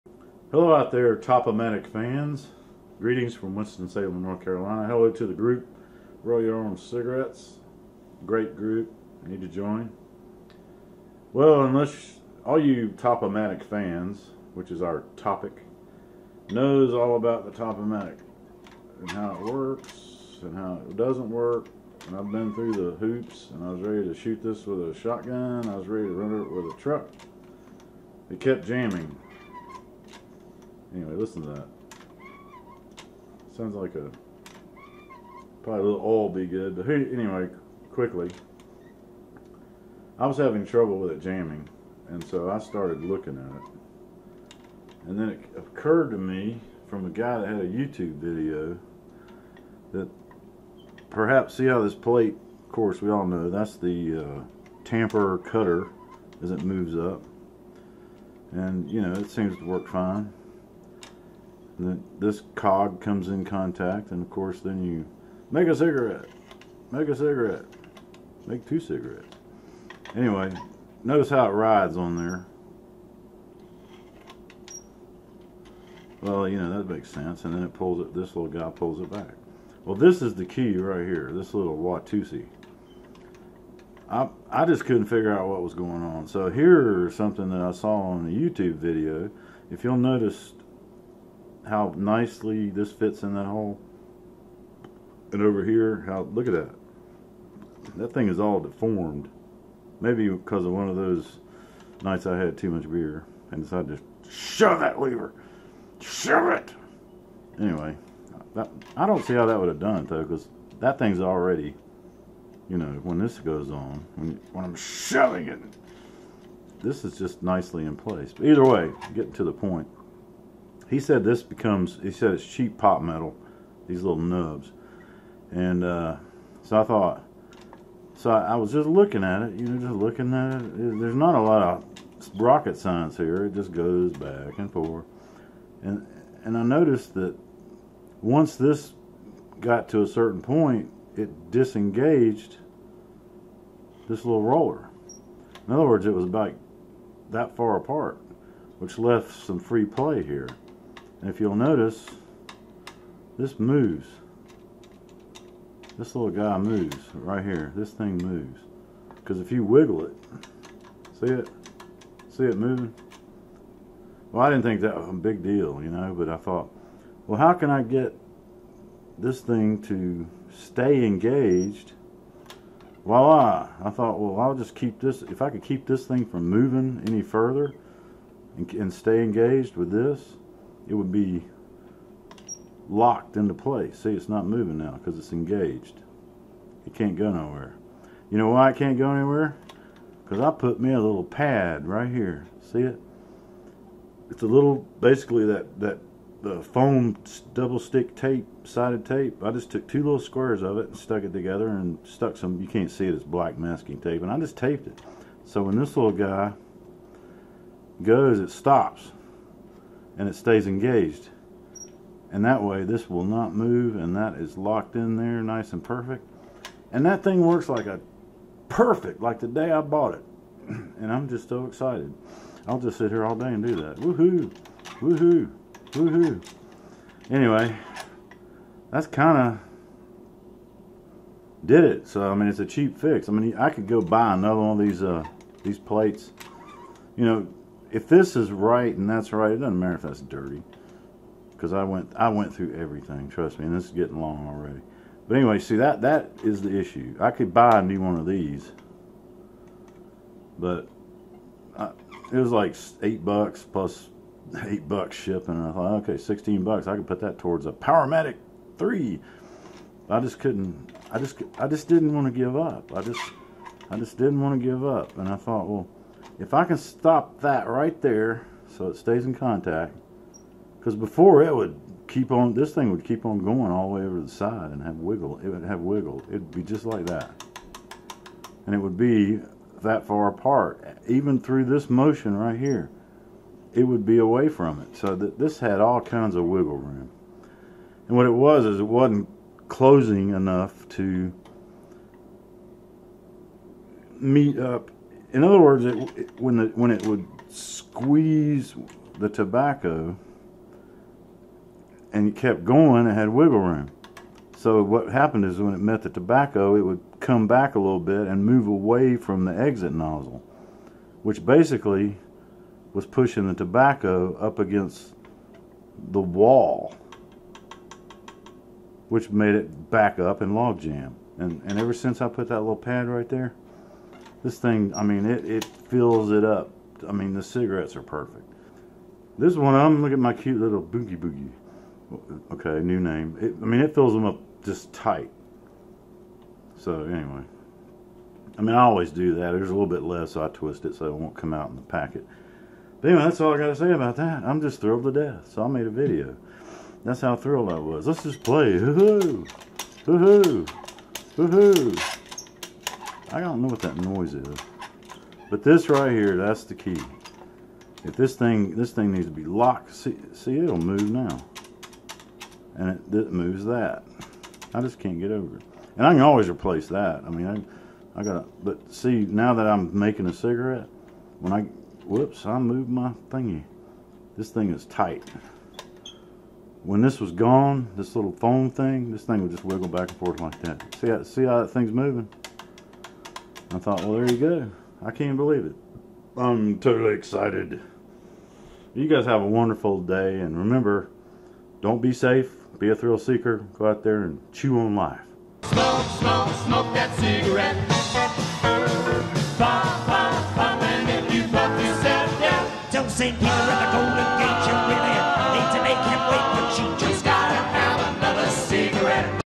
Top O Matic FIX
If you are at your wits end and ready to toss your Top O Matic cigarette maker in the trash...try this!  I was having constant jams with all tobacco.  Now it works beautifully!  Hope this helps!